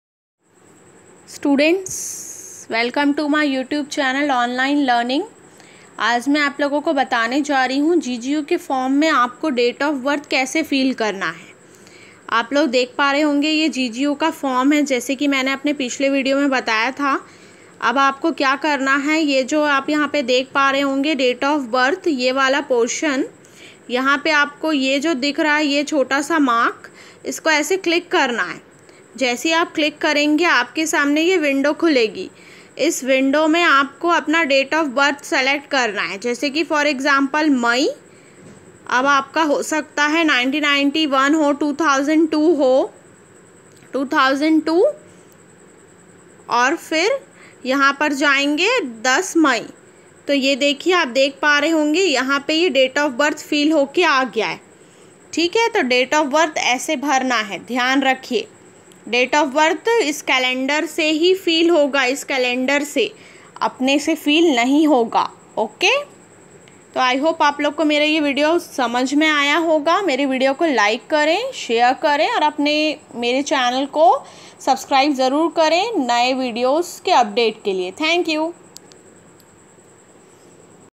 How to fill Date of Birth in GGU Form (Hindi/English)
Hello Students

Aaj hum aap ko iss video me "How to fill Date of Birth in GGU Form (Hindi/English)" Ke bare me batne ja rahe hai

How to resize passport photo and signature for any online form:-